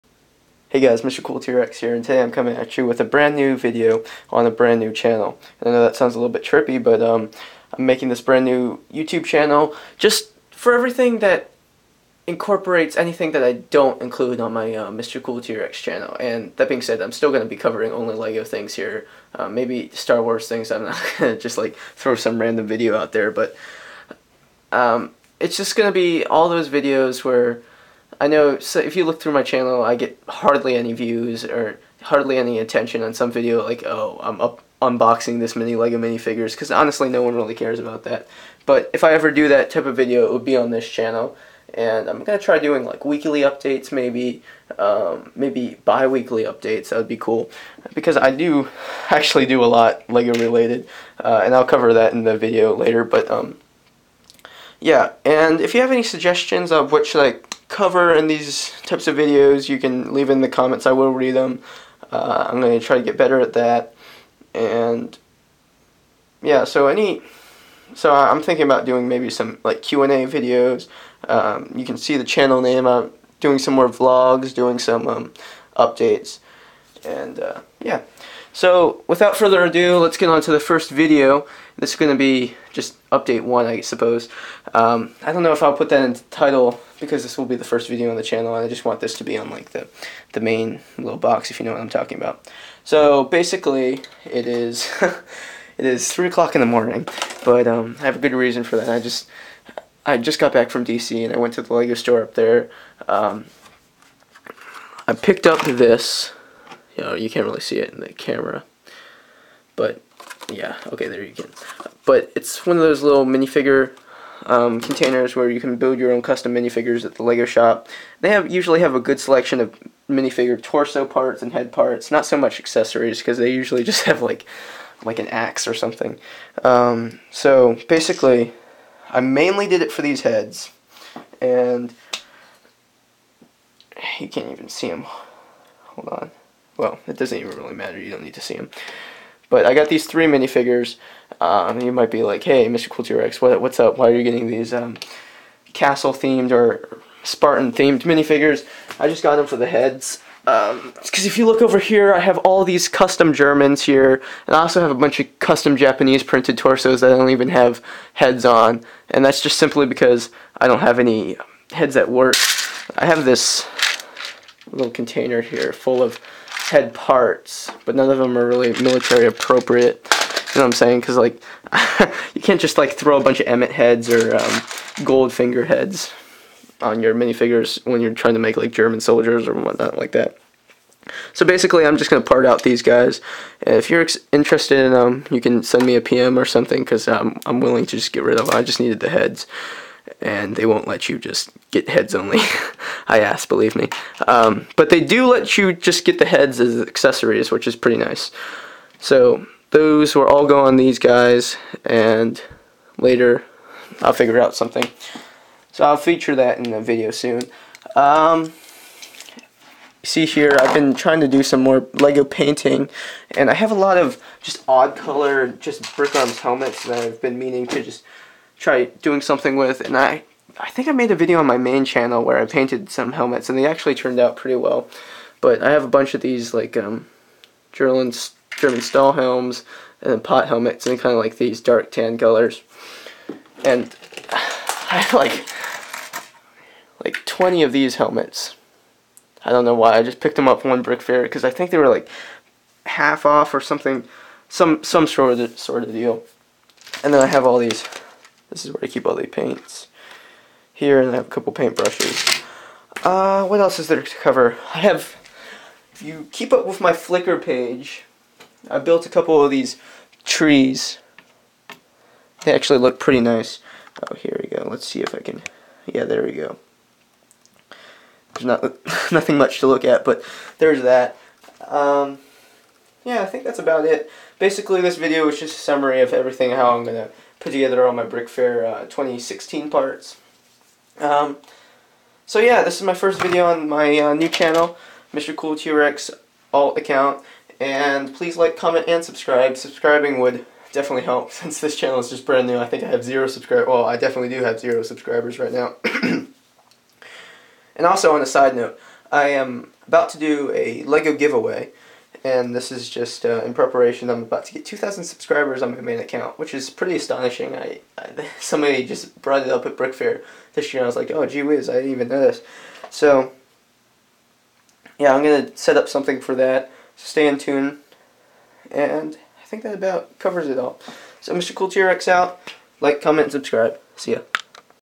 First Video, Gathering Parts for WW2 Figs (Vlog #1)
Hey guys this is the *authentic* mrcooltrex here...  Basically, I have created a separate channel to allow more frequent updates and to allow a more diverse range of content videos.